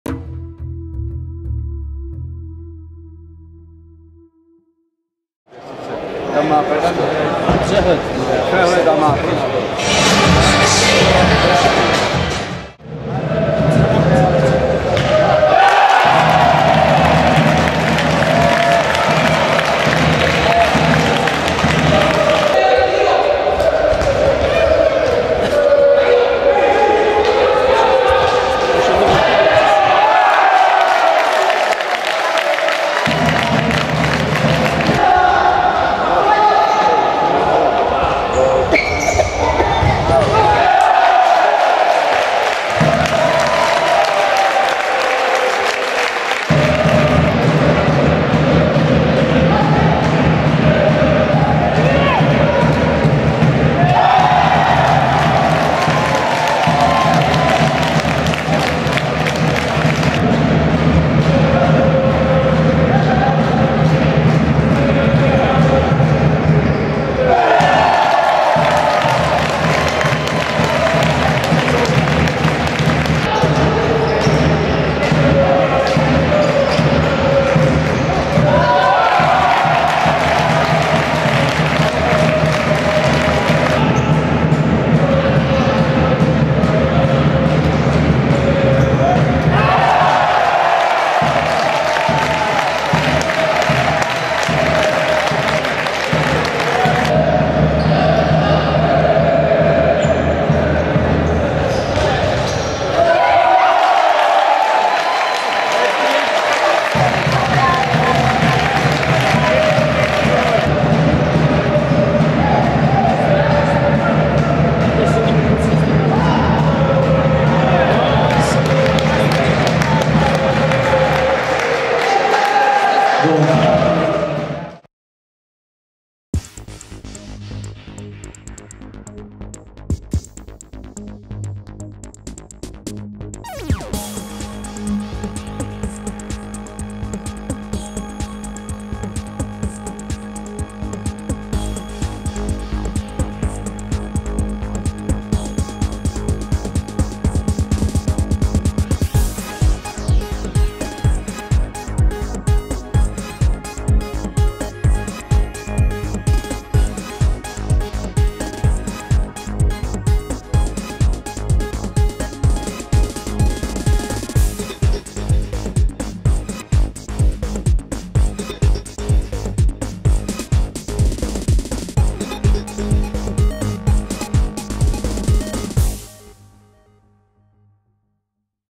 FC Balticflora Teplice - AC Sparta Praha 8:1 (2:0)
III. zápas semifinále play off CHANCE futsal ligy 2014/2015